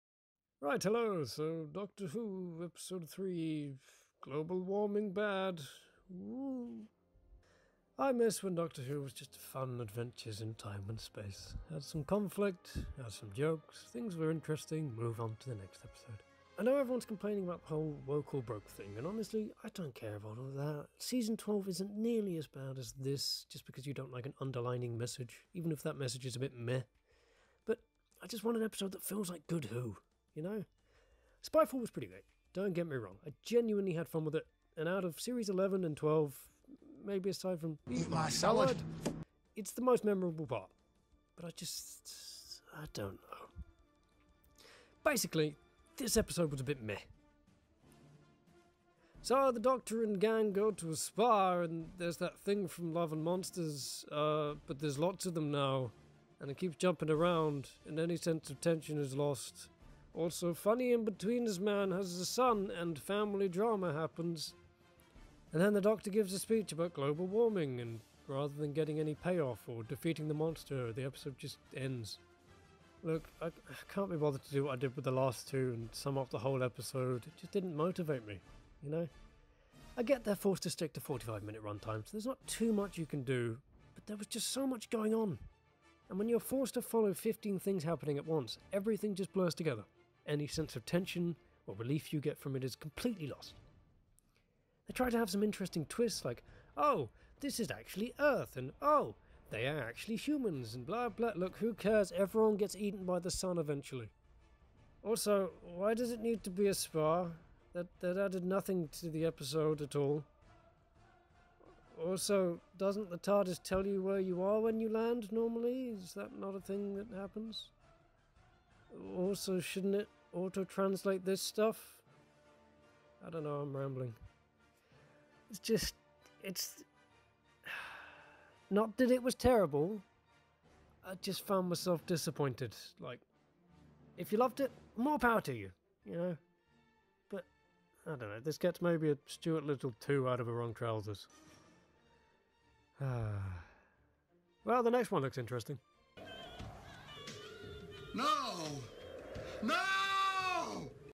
Doctor Who Orphan 55 | Review
not so great unfortunately, going steadily downhill but hopefully episode 4 will be better